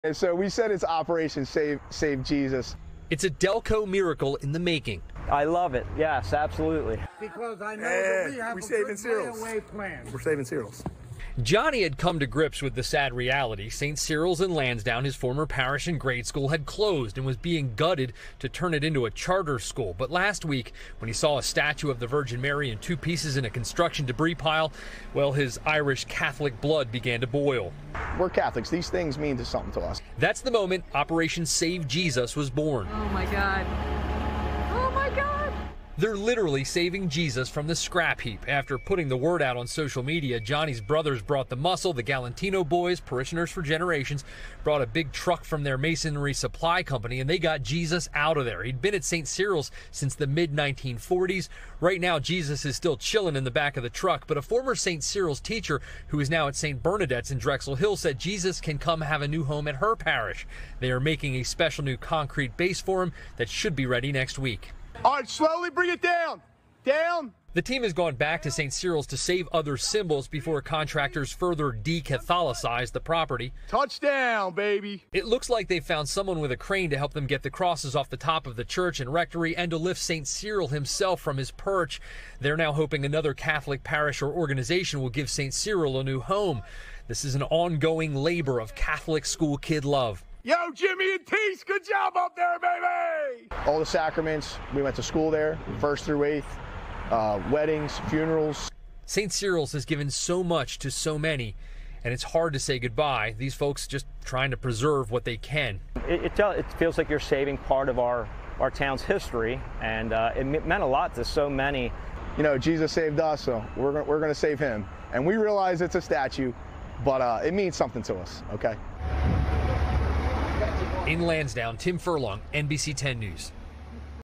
Operation ‘Save Jesus' Rescues Jesus Statue From the Scrapyard | NBC10
A group of Catholics are banding together to save the statues of Jesus and other religious figures from being destroyed along with their former parish in Delaware County. NBC10’s Tim Furlong has the details.